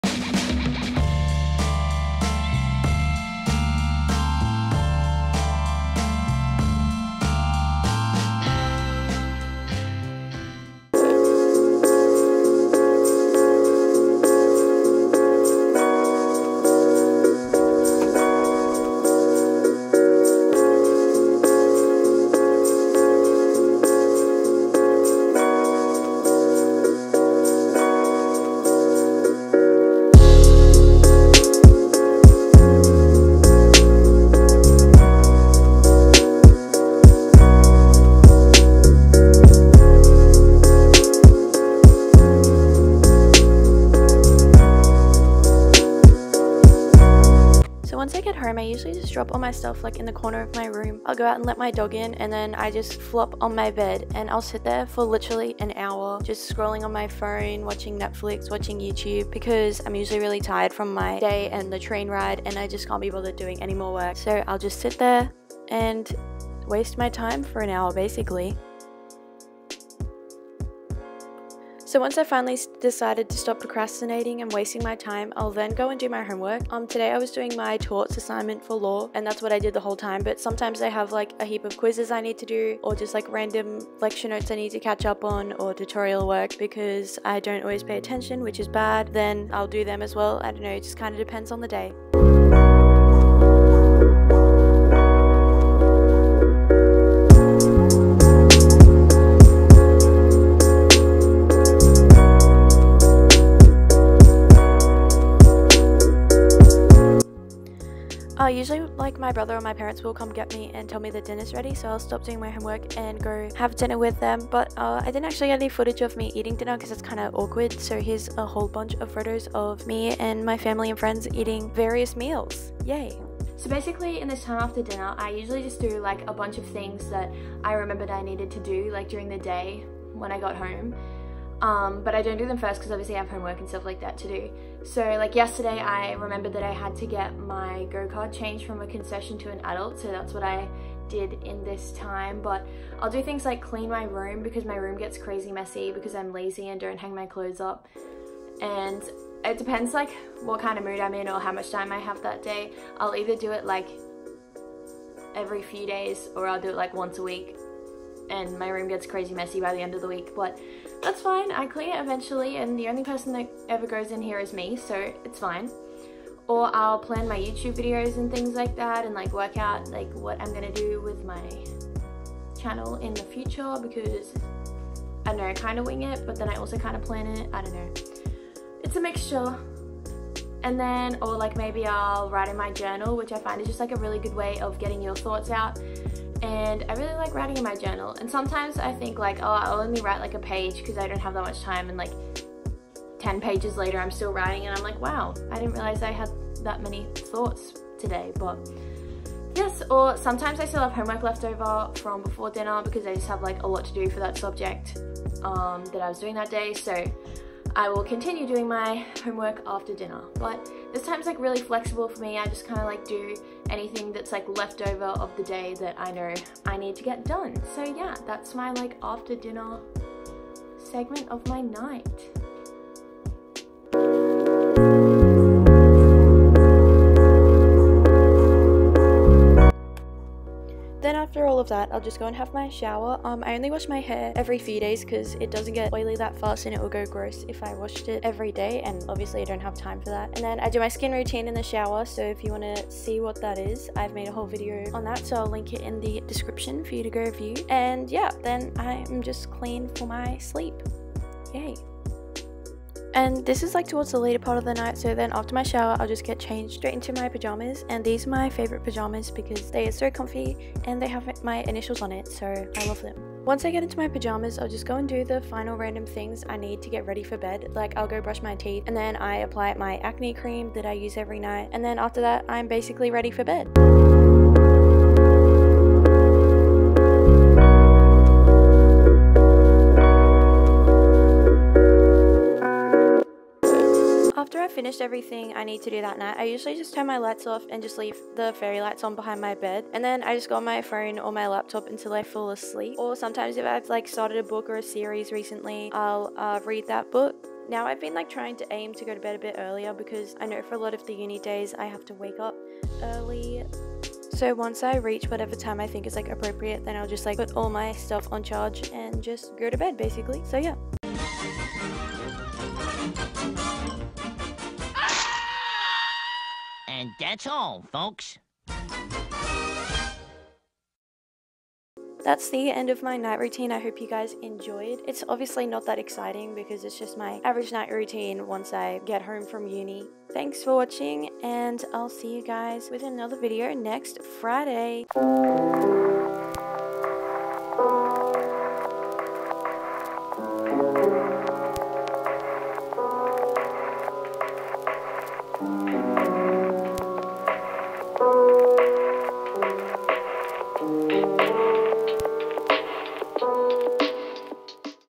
typical uni student night routine 2019
hey guys!! here’s my uni night routine for this year. the timing of all this really depends on the day and whether all my classes are on since that changes what time I get home. thank you for watching and I hope you enjoying my cliche night routine.

jess x